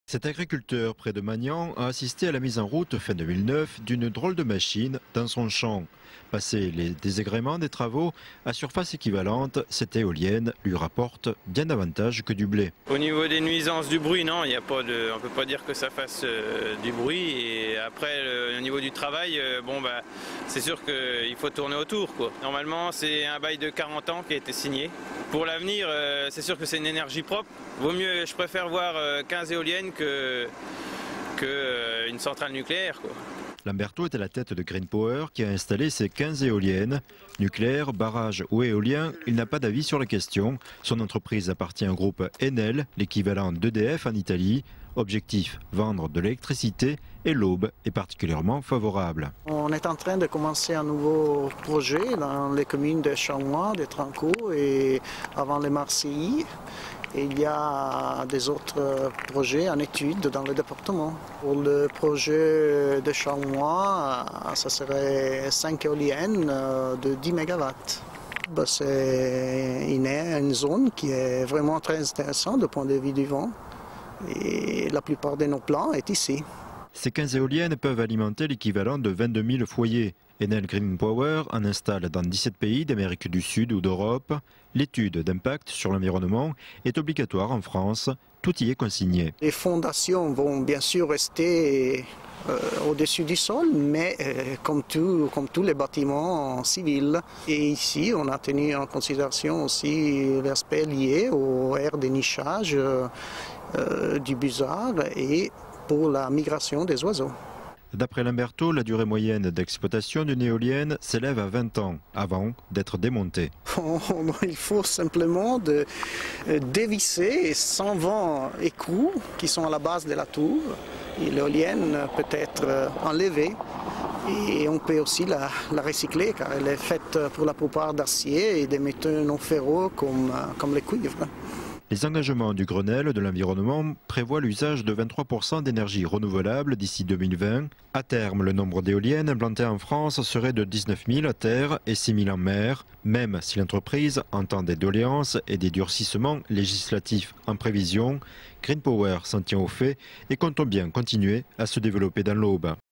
L'Aube : L'Eldorado pour les Eoliennes ?
15 nouvelles éoliennes ont été installées dans l'Aube, une source d'énergie alternative qui tend à se développer dans l'aube. (DROITS RESERVES . Pour toute exploitation sur TV, web, mobile, aller